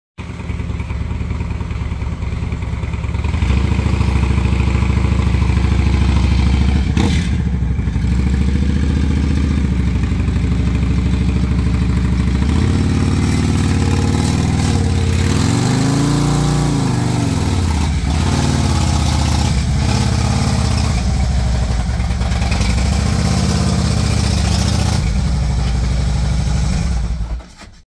Bolt-it-on Wheel chock Toy hauler edition
This was my 1st attempt to load my 2016 Harley-Davidson Road Glide fat tire into my 2023 Forest River 18rjb wolf pup.

2016 Harley Davidson Road Glide Special